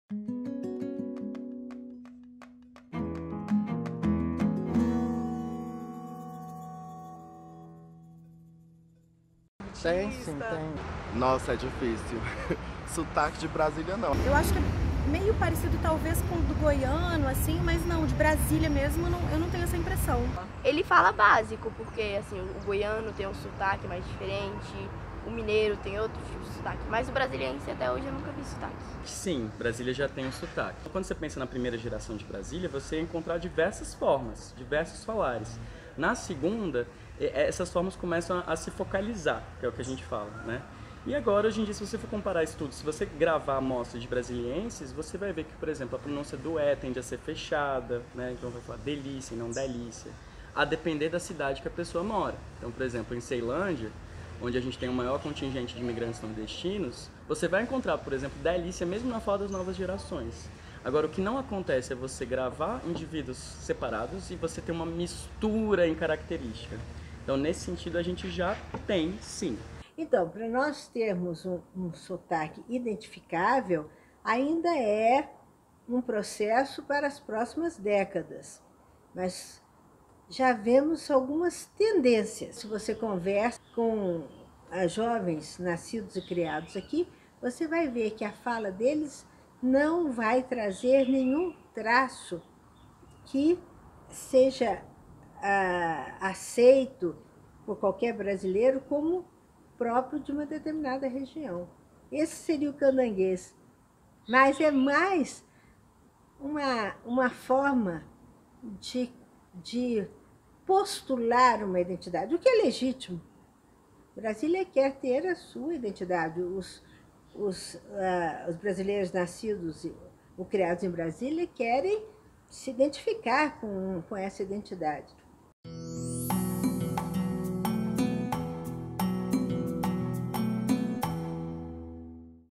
Brasília, 60 anos: o sotaque da Capital Federal
Uma população unida há 60 anos já viveu o suficiente para se expressar em um único sotaque? Fomos às ruas conversar com brasilienses e especialistas.